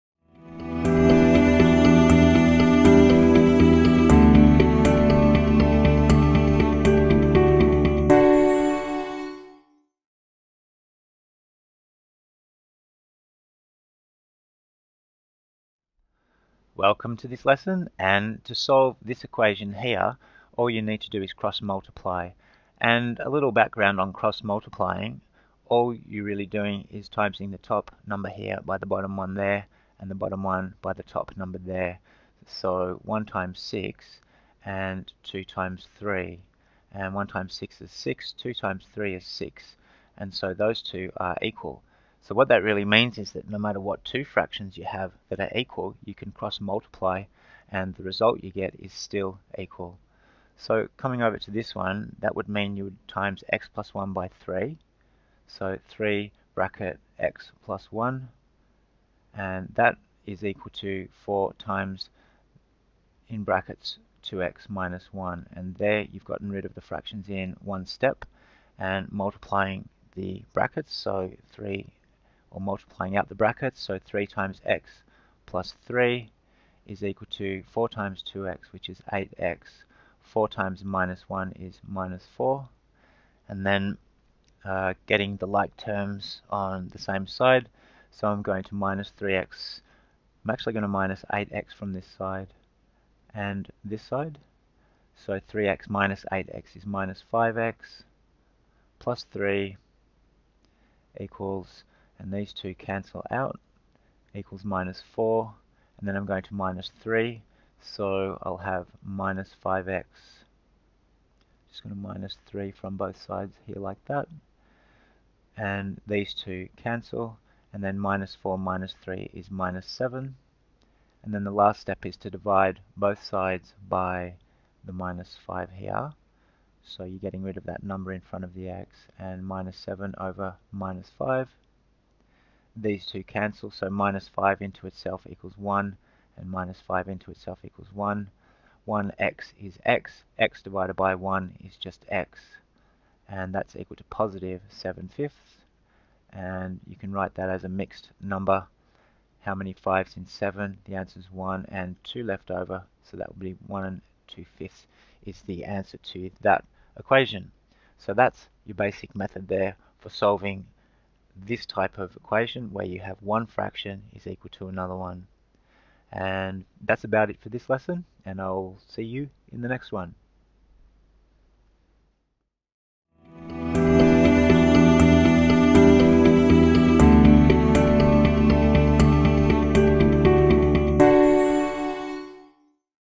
How to Cross Multiply Fractions with variables
In this lesson you'll learn you'll learn How to Cross Multiply Fractions to Solve for X. The equation is made up of one fraction that's equal to another fraction and the simplest way to get started is by cross multiplying. fraction.